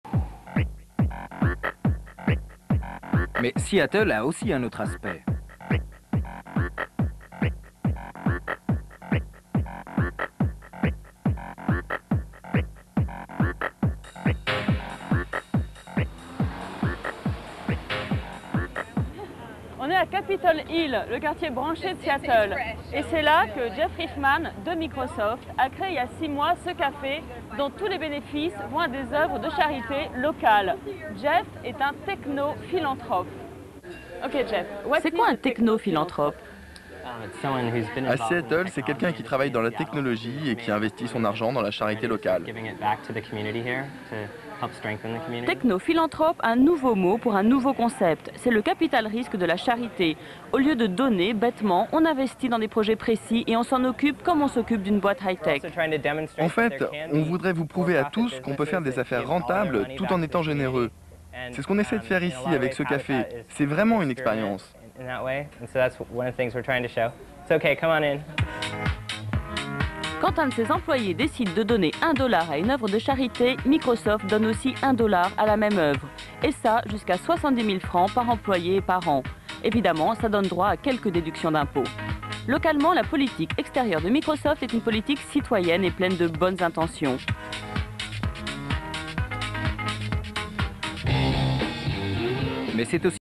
Canal+ on Habitat Espresso
French station Canal+ Interviews Jeff Reifman on Habitat Espresso and Microsoft